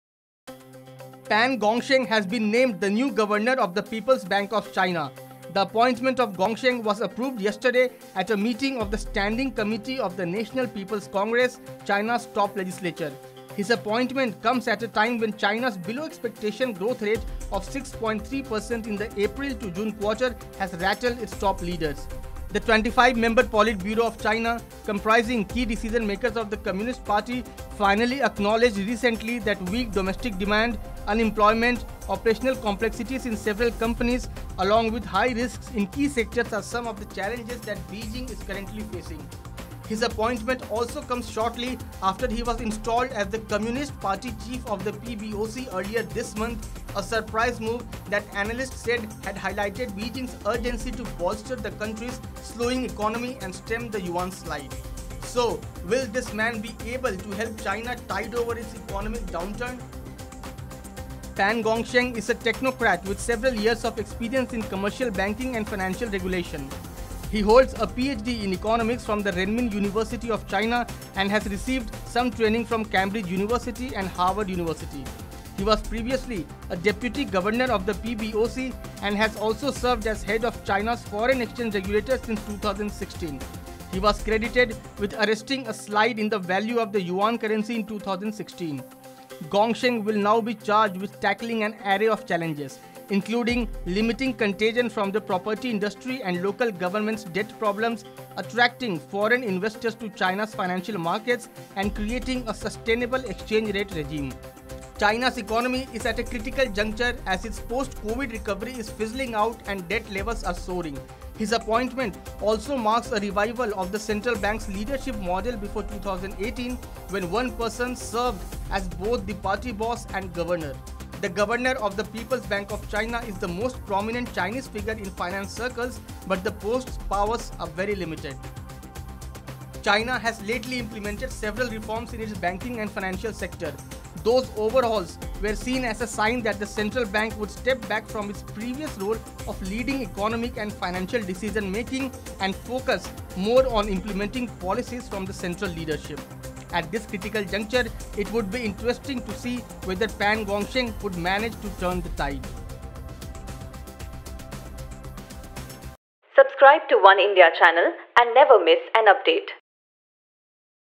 Pan Gongsheng: China throws a surprise by appointing him as the Central Bank's Chief | Oneindia News
Pan Gongsheng has been named the new governor of the People’s Bank of China. The appointment of Gongsheng was approved yesterday at a meeting of the Standing Committee of the National People’s Congress, China’s top legislature.  His appointment comes at a time when China’s below-expectation growth rate of 6.3 % in the April-June quarter has rattled its top leaders. The 25-member Politburo of China, comprising key decision makers of the Communist Party, finally acknowledged recently that weak domestic demand, unemployment, operational complexities in several companies…along with high risks in key sectors are some of the challenges that Beijing is currently facing. His appointment also comes shortly after he was installed as the Communist Party chief of the PBOC earlier this month, a surprise move that analysts said had highlighted Beijing’s urgency to bolster the country’s slowing economy and stem the yuan’s slide. So, will this man be able to help China tide over its economic downturn?